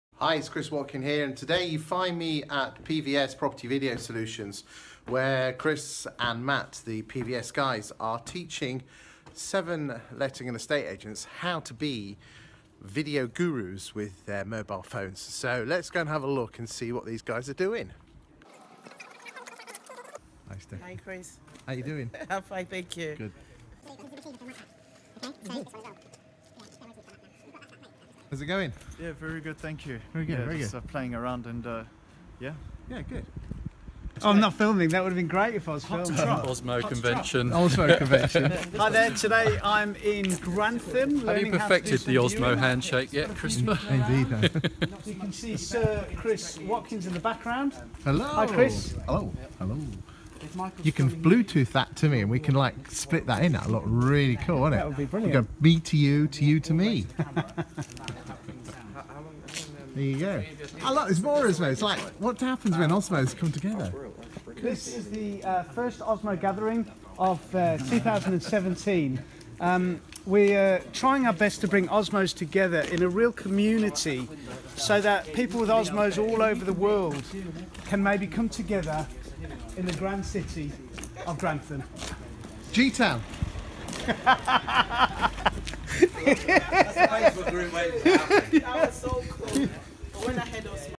Behind the scenes at Video Training in your Estate Agency Pt2
Friday, 15th September 2017 and myself and the Property Video Solutions boys are teach letting and estate agents how to use video